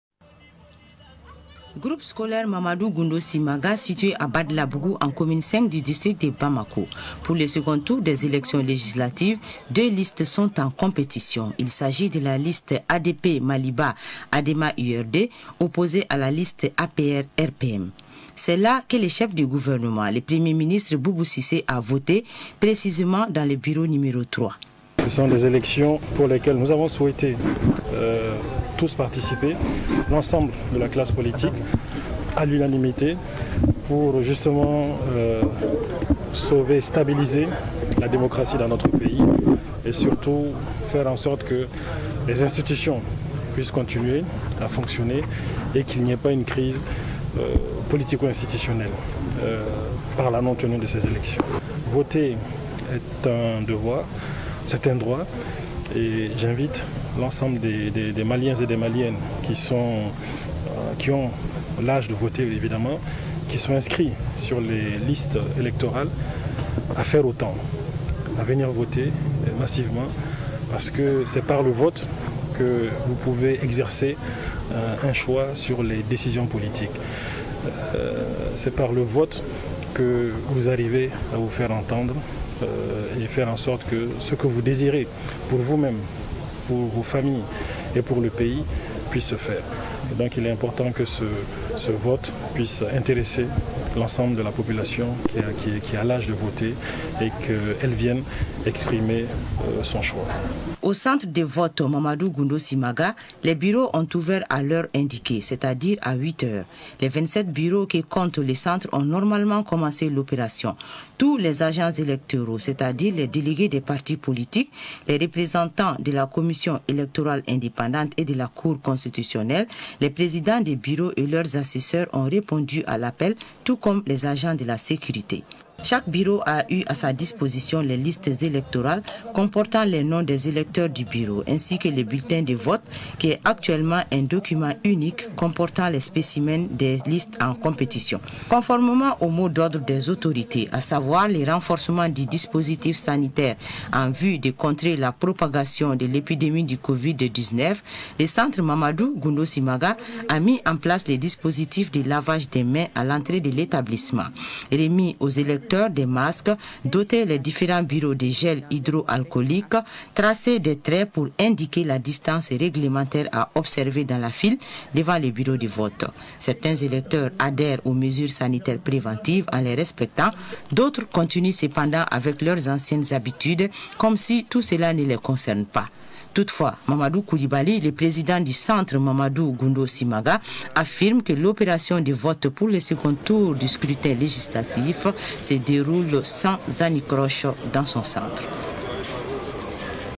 Législatives2020 StopCoronavirusML🇲🇱 Le premier Ministre Dr Boubou Cissé a accompli son devoir
Le premier Ministre Dr Boubou Cissé a accompli son devoir citoyen ce matin au bureau de votre n°003 du centre de vote de l'École fondamentale Mamadou Simaga de Badalabougou.

 Reportage ORTM1